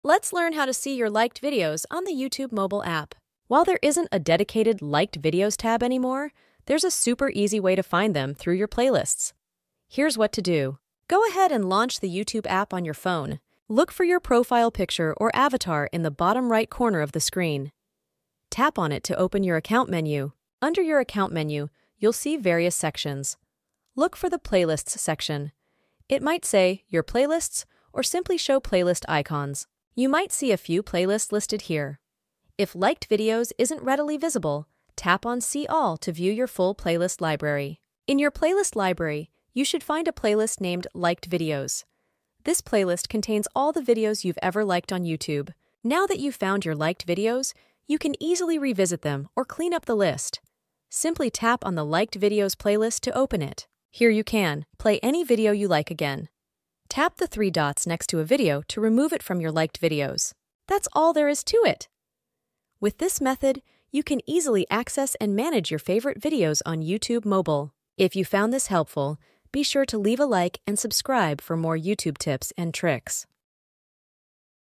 How To See Liked Videos On YouTube (Step by Step)
Learn How To See Videos you Liked On YouTube (Step by Step)
Ever wondered how to access your liked videos on YouTube? Look no further! In this step-by-step tutorial, we'll walk you through the process of finding and managing your liked videos on the platform. Whether you're trying to revisit a favorite clip or organize your saved content, understanding this feature is essential. Join us as we unveil the secrets to accessing your liked videos effortlessly. Don't miss out—watch now and unlock a world of personalized content at your fingertips. 🚀🔍